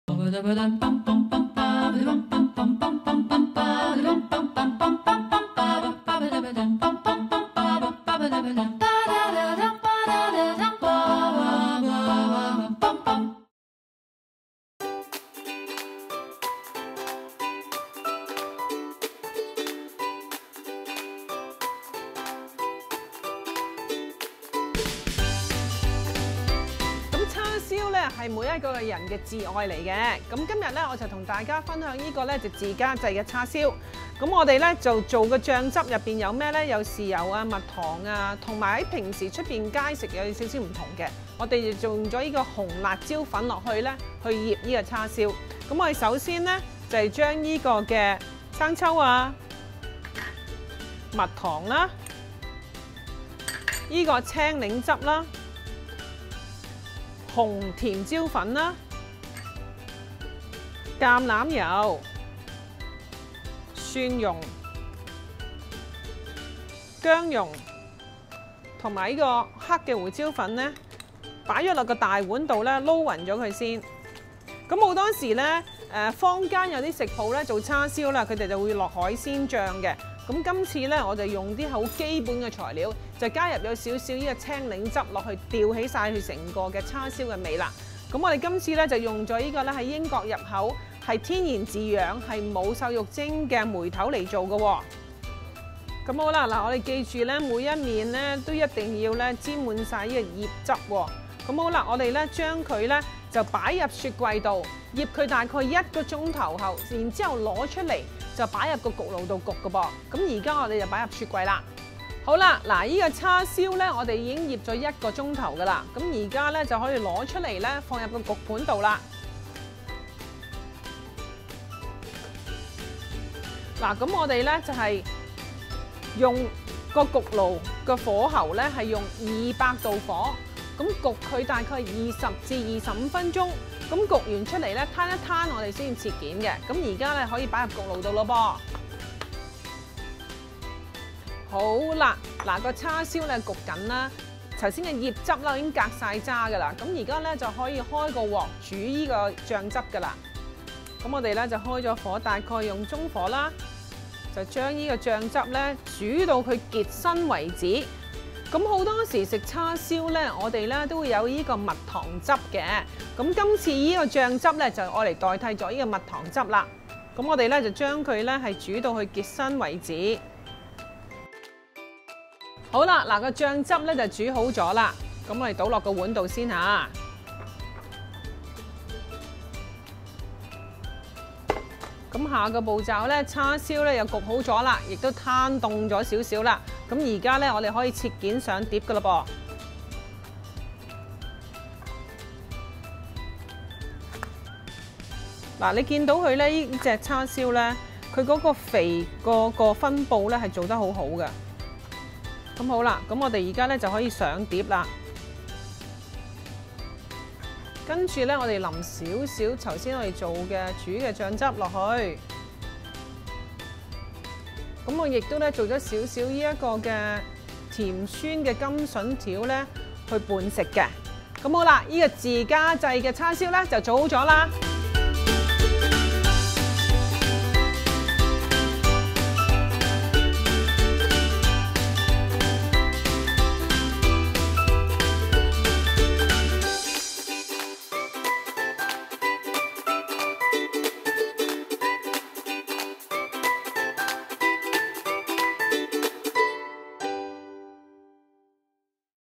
【Denice星級廚房】第11集 - 自家制叉燒 Homemade Oriental BBQ Pork
自家制叉燒食譜

Waveney Valley梅頭 1條約1磅半
莞茜碎裝飾
豉油  5湯匙
蜜糖  4湯匙
青檸汁  4湯匙
橄欖油  1湯匙
蒜茸  1湯匙
紅甜辣粉 半湯匙
薑茸  半湯匙
黑胡椒粉 ¼ 茶匙

1. 將豉油, 蜜糖, 青檸汁, 紅甜辣粉 , 橄欖油 , 蒜茸, , 薑茸, 黑胡椒粉 在碗內拌勻,
2. 梅頭放入保鮮袋, 倒入醃汁; 放入雪櫃醃1小時或過夜
3. 已醃好梅頭取出; 餘留醃汁;
4. 將梅頭放入200C焗爐焗20 - 25 分鐘或至全熟; 待頭10分鐘反轉焗另一面; 從焗爐取出, 待5分鐘後切片
5. 醃汁倒入鍋內煮滾後調慢火煮5分鐘; 熄火
6. 叉燒件上碟淋上醬汁灑上莞茜碎

小貼士
1. 可用豬柳除代梅頭
2. 選擇半肥瘦梅頭可令菜式更美味可口

Homemade Oriental BBQ Pork

1 ½ lbs Weavney Valley Collar Butt
chopped fresh cilantro, for garnish
1/6 cup soy sauce
1/8 cup honey
1/8 cup fresh lime juice
1 tablespoons olive or vegetable oil
2 cloves garlic, finely chopped
1/2 tablespoon curry powder
1/2 tablespoon grated fresh ginger
1/4 teaspoon ground black pepper

1. In a small bowl, combine soy sauce, honey, lime juice, oil, garlic, curry powder, ginger and black pepper; mix until well blended.
2. Place collar butt into the plastic bag, pour marinade over.
3. Close bag tightly and marinate in refrigerator for 1 hour or up to overnight.
4. Remove meat from marinade; reserve marinade.
5. Set oven to 200C, roast for 20 – 25 minutes or until is meat is fully cooked, turning after first 10 minutes. Remove from oven and let side for 5 minutes then slice into pieces
6. Boil reserved marinade in a small saucepan, reduce heat and simmer 5 minutes. Serve with sliced pork, garnish with cilantro.

Tips:
could use pork tenderloin instead of collar butt
use medium fat collar butt as it gives more aroma and favour to the dish